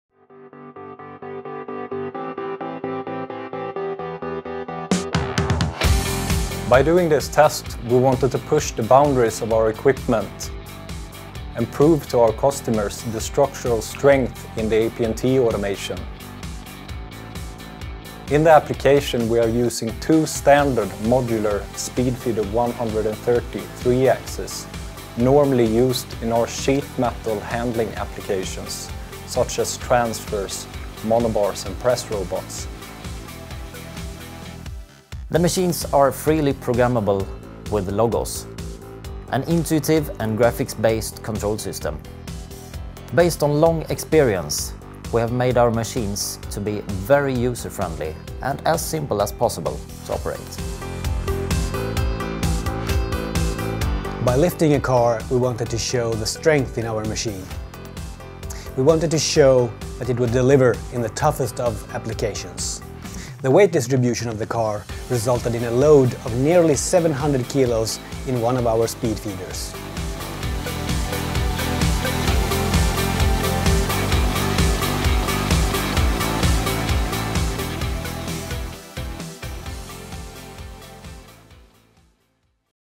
AP&T standard automation vs. fully equipped car
One AP&T standard automation. No additional construction and no fake footage. We wanted to show the durability of our modularized automation solution by lifting and moving a fully equipped car at our production facility in Ulricehamn, Sweden.